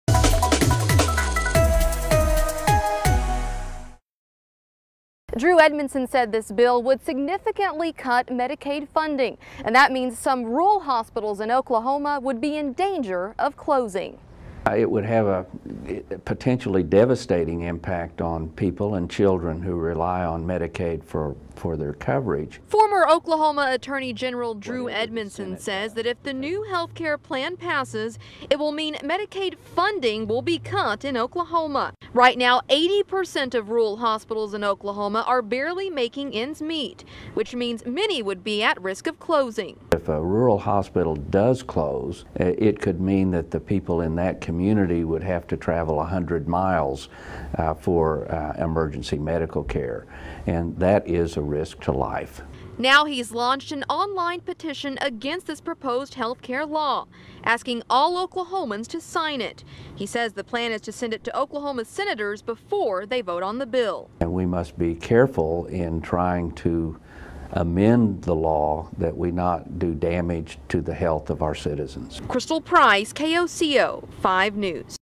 New health care bill could put rural Oklahoma hospitals in jeopardy
Some people have spoken out against a proposal to cut Medicaid funding, and a former Oklahoma attorney general has even started a petition against it.